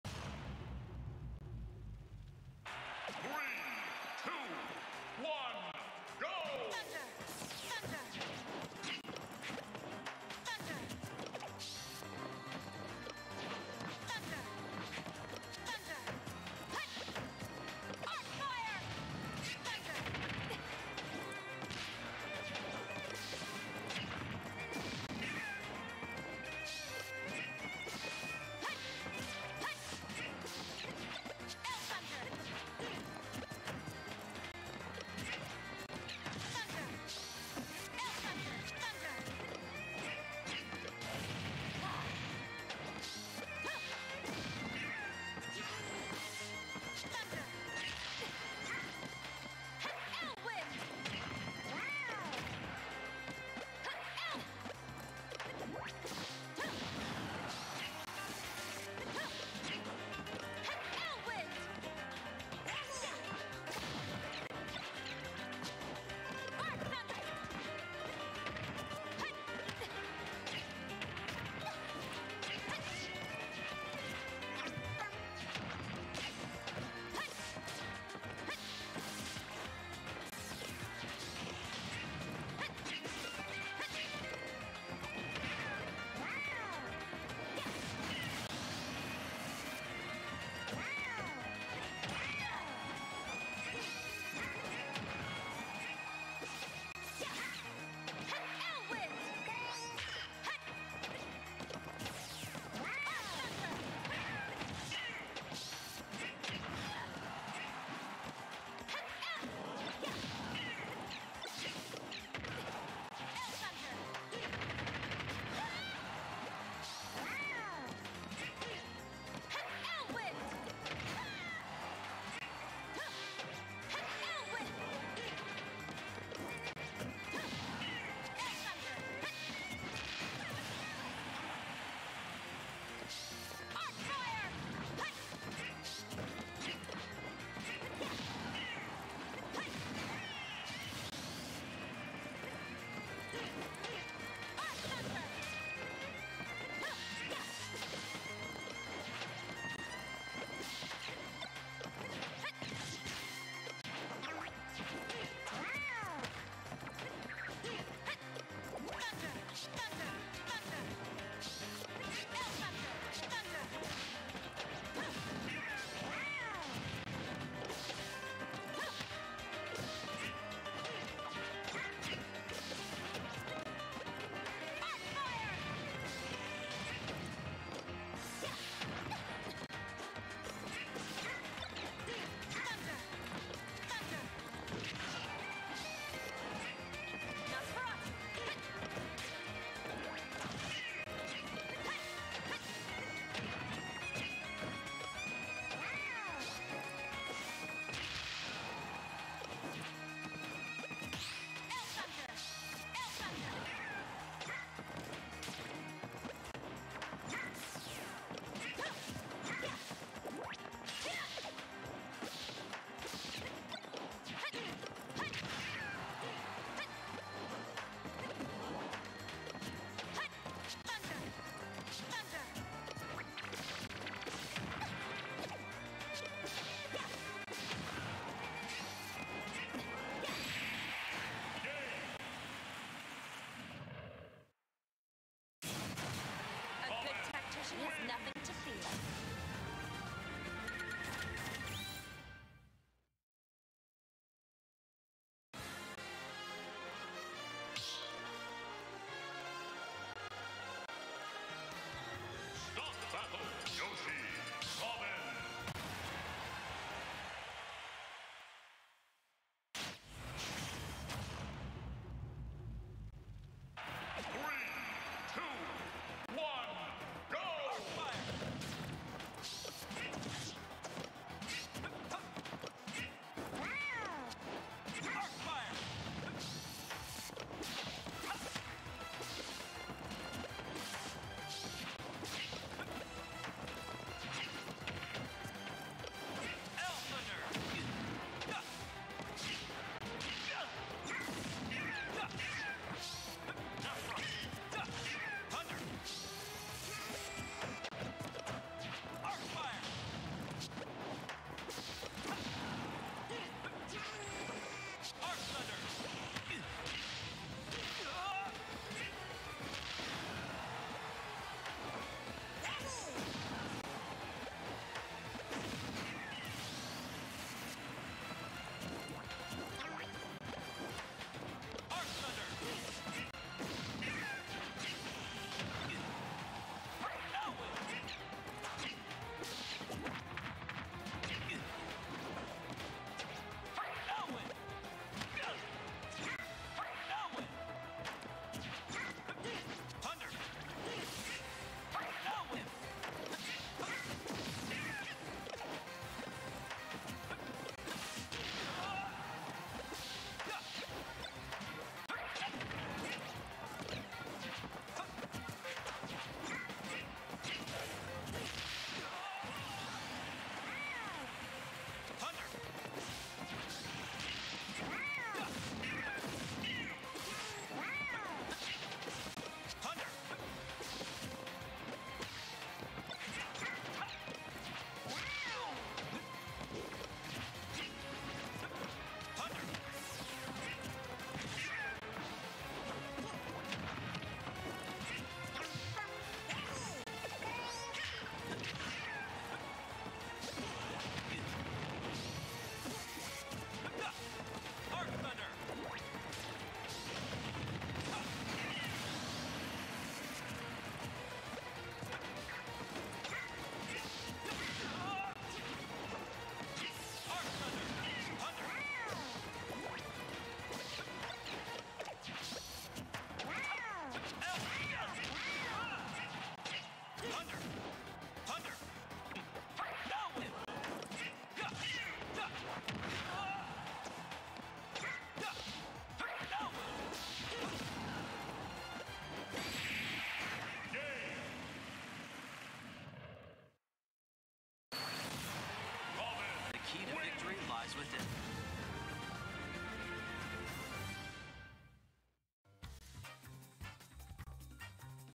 Bmm (Robin) vs Raptor (Yoshi) | AONUltimate #121 Winners Round 3
"Bmm vs Raptor | AONUltimate #121 Winners Round 3 | AONUltimate #121 is presented by GenGAME, The Meta: Gaming and esports, and AON Gaming.

Produced By: AFWagers @AFWagers
Executive Director: Tmags @themags"